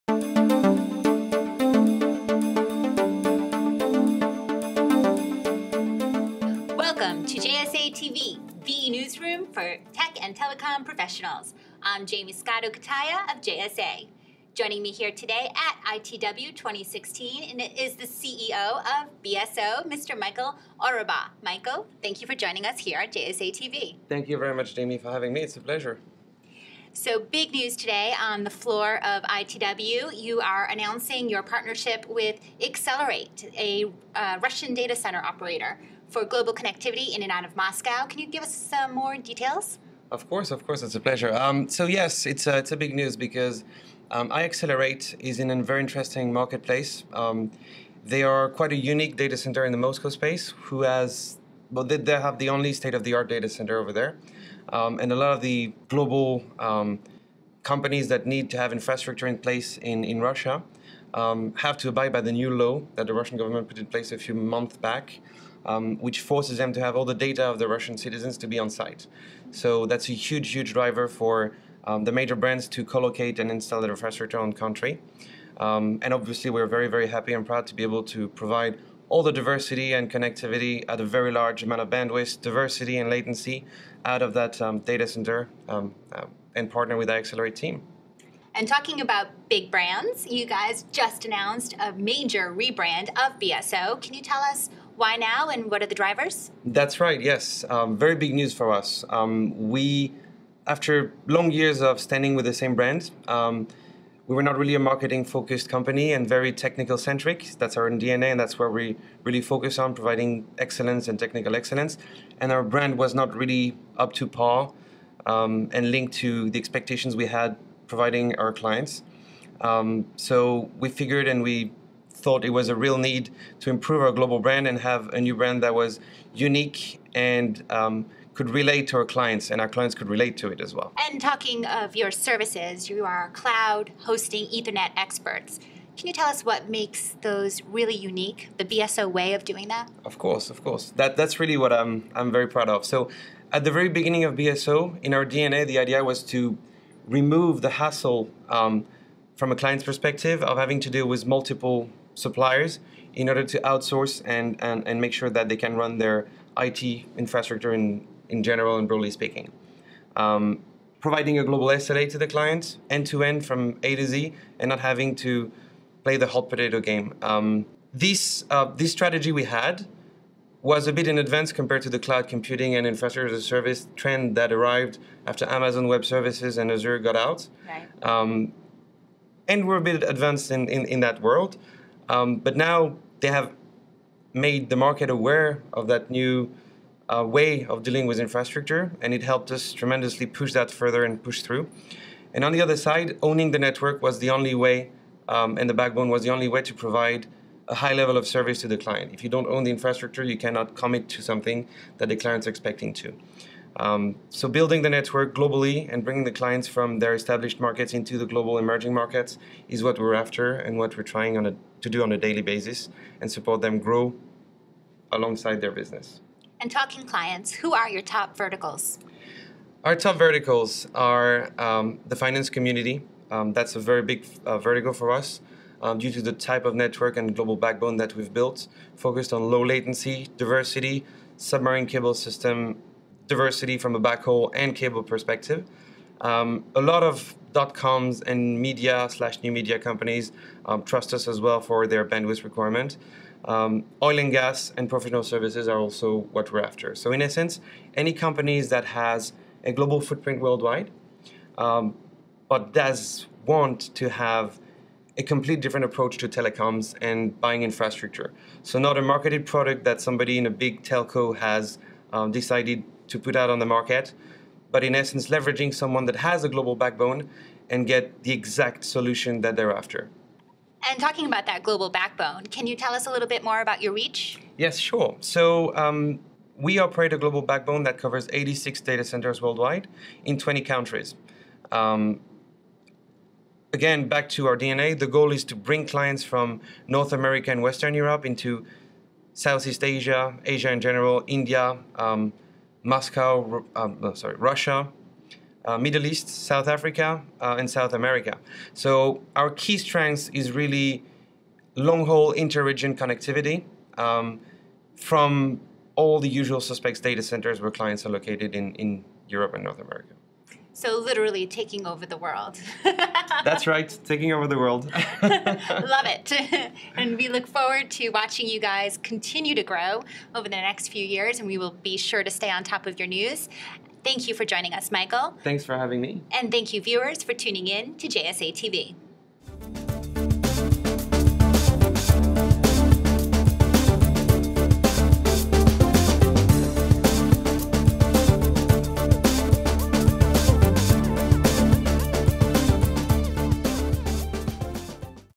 BSO CEO Talks to JSA TV on Company Rebrand, Partnership with IXcellerate
Michael Ourabah, Chief Executive Officer of BSO, the Ethernet network, cloud and hosting provider of choice for global businesses wanting superior technology infrastructure and connectivity to the world’s most dynamic marketplaces, discusses the company's latest updates on JSA TV at ITW 2016. BSO recently announced its partnership with IXcellerate, Russia’s premier carrier-neutral data center operator, to improve fiber access into and out of Moscow, home to Russia’s (MOEX) Exchange. Mr. Ourabah also talks about the company's recent rebrand, aiming to improve its global brand, and have a brand that was unique and relate to its clients. Watch the video to learn more about the "BSO Way," and upcoming company initiatives.